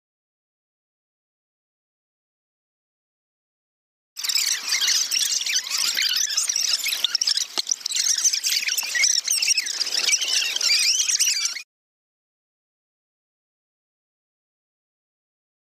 Banana slug eating time lapse
Banana Slug eating a carrot in a cup at camp in 2010! What a special moment